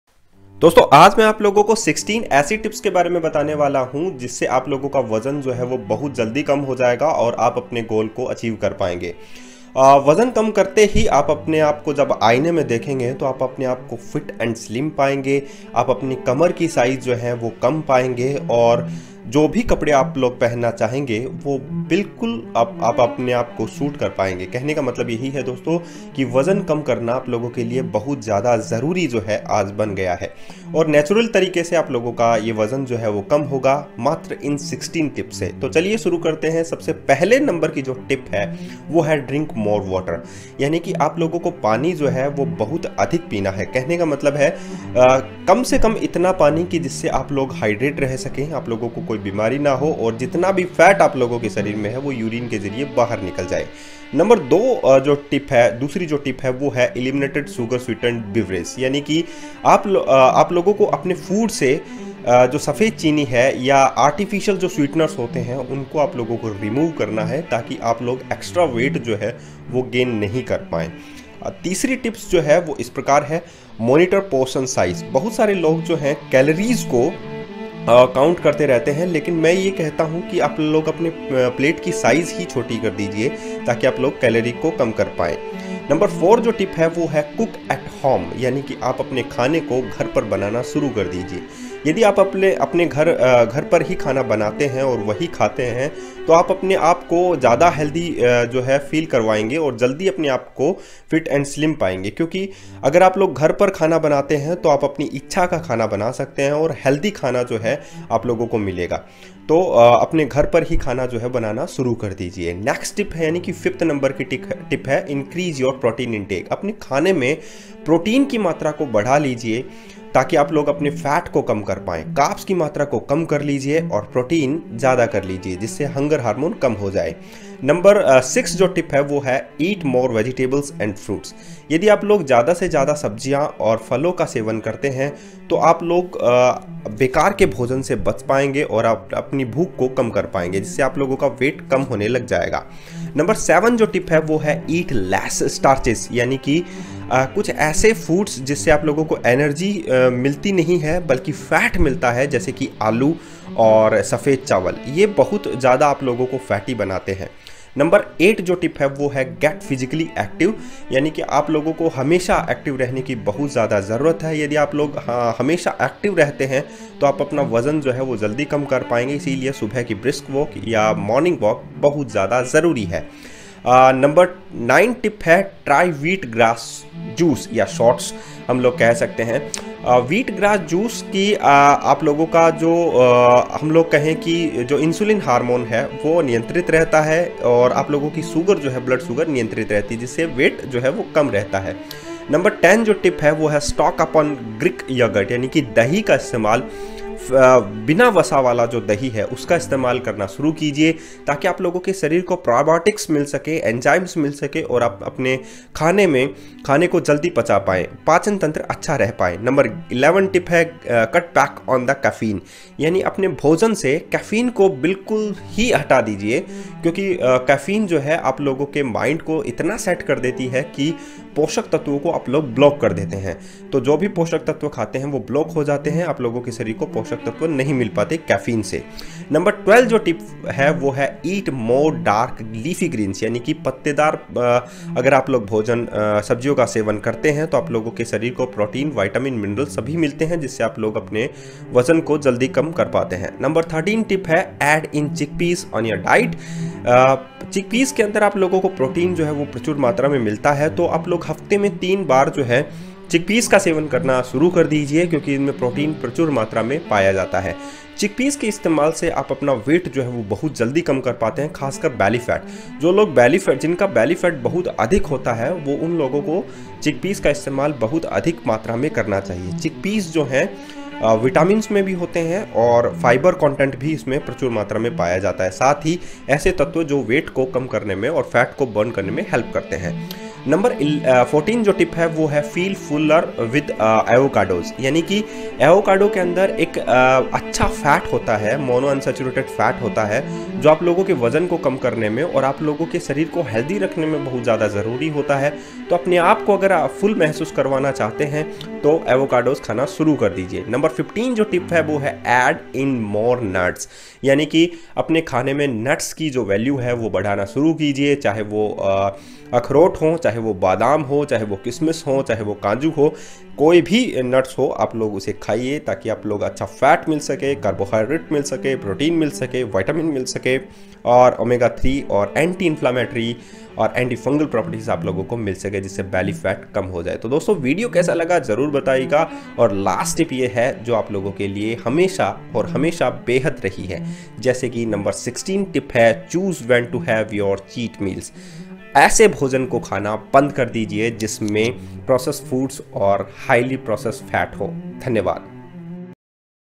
तेजी से वजन कम करने की 16 टिप्स जो साइंस के आधार पर सत्य हैं | Weight Loss 16 tips
1. Drink more water
2. Eliminate sugar-sweetened beverages
3. Monitor portion sizes
4. Cook at home
5. Increase your protein intake
6. Eat more vegetables and fruits
7. Eat less starches
8.  Get physically active
9. Try wheatgrass shots
10. Stock up on Greek yogurt
11. Cut back on the caffeine
12. Eat more dark, leafy greens
13. Add in chickpeas to your diet
14. Feel fuller with avocados
15. Add in more nuts
16. Choose when to have your "cheat" meal
Thank You So Much By : Ultimate Health and Home Remedies
Check out new videos of Ultimate Health and Home Remedies